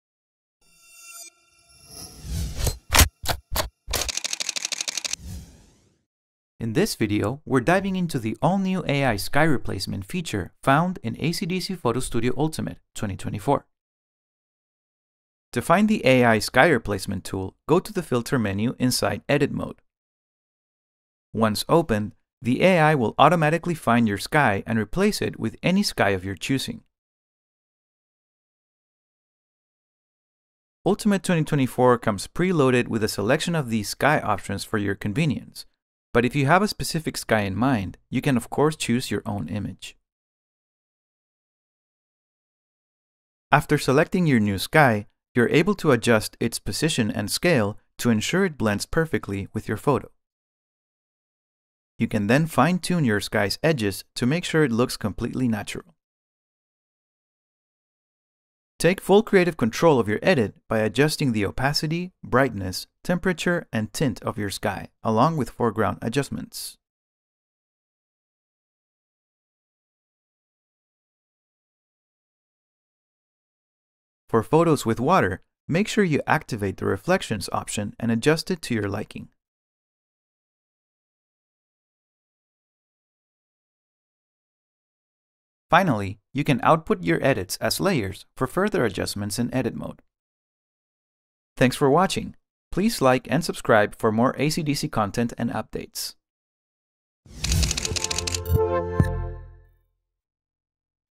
ACDSee Photo Studio 2024 - AI Sky Replacement
The sky is no longer the limit with AI Sky Replacement. Easily define and replace the sky in any photo, resulting in a natural-looking composition thanks to foreground and reflection options for water. You can also use one of the many pre-installed skies, like Sunset, Storm, and Night Sky, to transform your wildest dreams into your horizon realities.

ACDSee Photo Studio Ultimate 2024 is the complete solution for photographers and creatives of all levels. The software comes fully loaded with new and improved features, boosted by Artificial Intelligence (AI), to help you organize, search, and edit your photos with minimal time and effort.